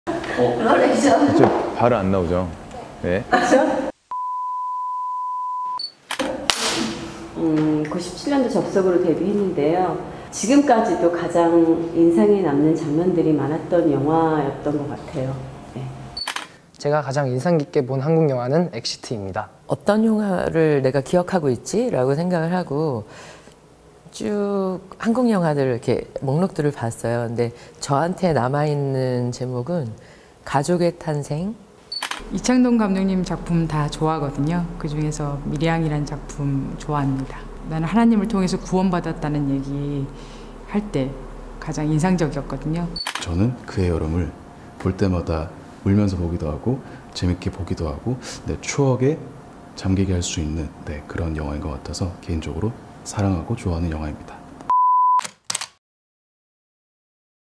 전도연 이 사랑한 한국영화는?
마리끌레르 부산국제영화제 특별판에 등장한 배우들에게 ‘가장 사랑하는 한국 영화’를 물었습니다.
두번째 영상에 등장하는 전도연, 안지호, 백현주, 염혜란, 장기용은 어떤 대답을 했을까요?
영상으로 만나 보시죠.
We asked all the actors in Marie Claire Korea BIFF Special Edition what their favorite Korean Film is.